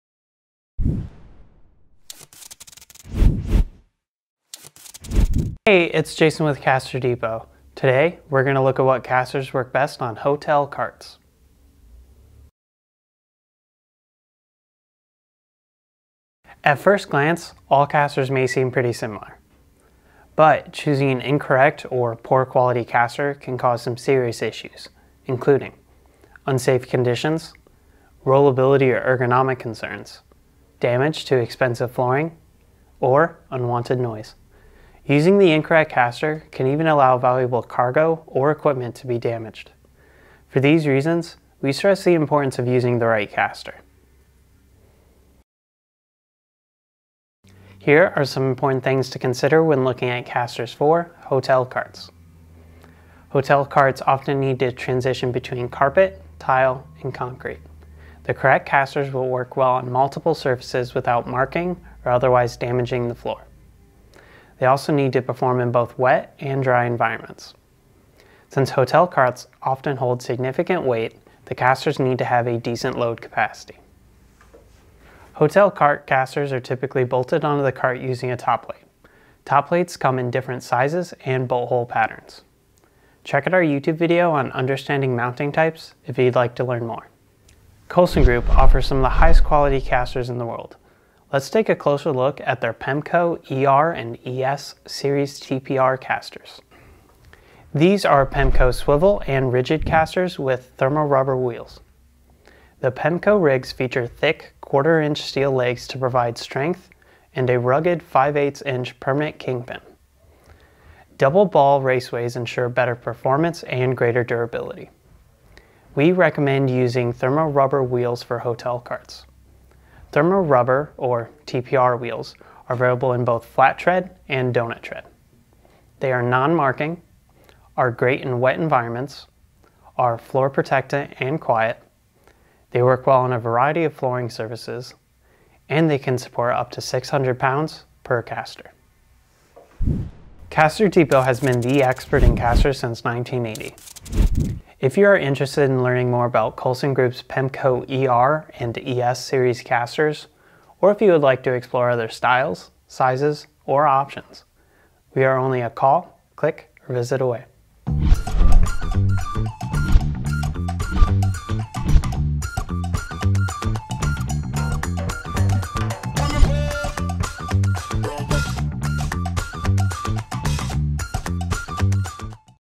Best Casters For Hotel Carts
See which Casters are best to use on hotel carts.
Things to consider for hotel carts:
- Ability to work across multiple flooring surfaces
- High performance in wet and dry conditions
- Adequate load capacity

We recommend using Colson's PEMCO ER and ES Series Casters with Swivel and Rigid Casters with Thermoplastic Rubber (TPR) Wheels

Some benefits are:
- Thick quarter inch steel legs to provide strength
- 5/8 inch permanent kingpin
- Strong and durable
- Available in both flat and donut thread
- Non-marking properties
- Work well in wet and dry environments
- Perform on multiple flooring surfaces
- Can support up to 600 lbs per caster

Correct casters and wheels can make the job easier, safer and faster. They can also protect flooring and valuable equipment. On the other hand, incorrect casters and wheels can cause some serious issues.

Since 1980, CasterDepot has been the expert in casters and wheels.

Follow us to get the latest updates and exclusive promos!

CasterDepot
Expert.
Easy.
Every Time.
ISO 9001 Certified QMS